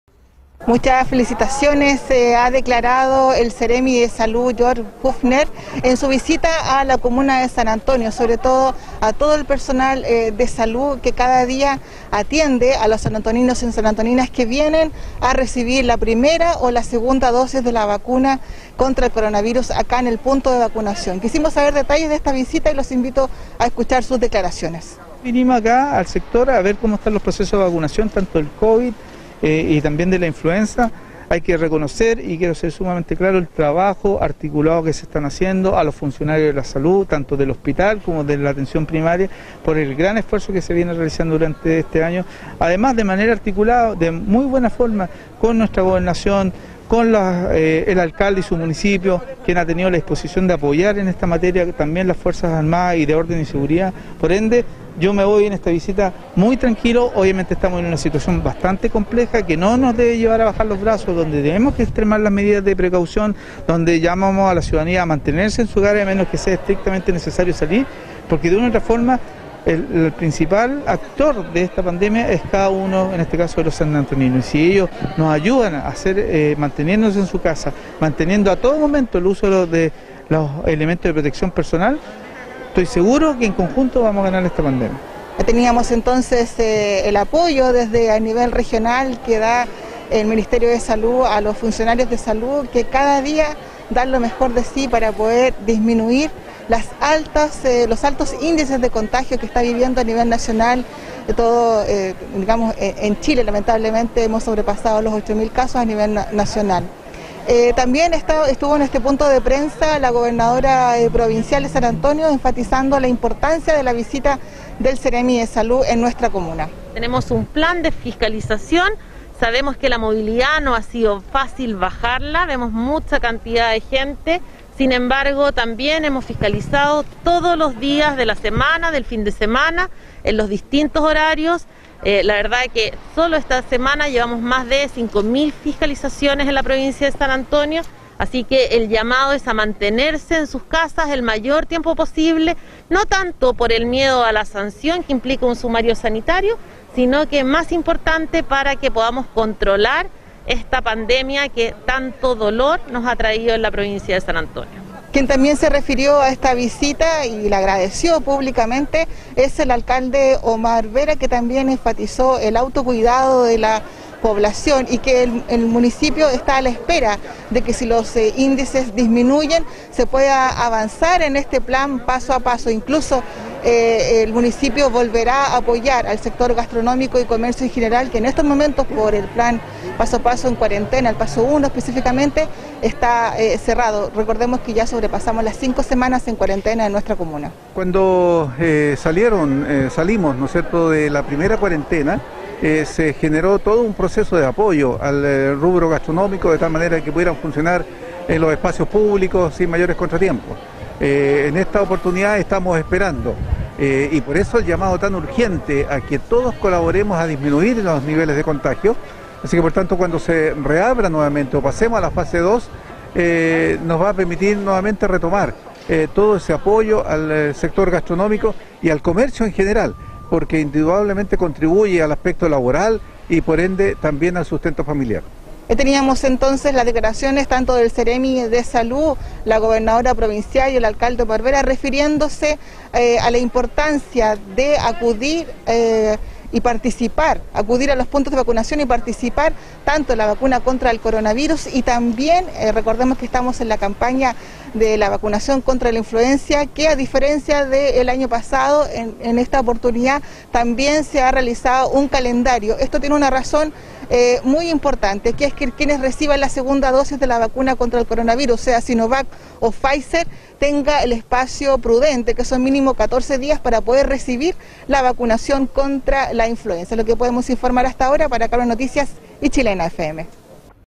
9 ABR 2021 San Antonio incluye nuevo test de Covid19
En visita a la comuna de San Antonio, el Seremi de Salud felicitó el trabajo de los funcionarios a la salud por la eficacia de la campaña “Yo Me Vacuno”. Además anunció el plan piloto de los test de antígenos enfocados a los pacientes con algún síntoma asociado a COVID19, este plan se desarrollará en el Cesfam Centinela.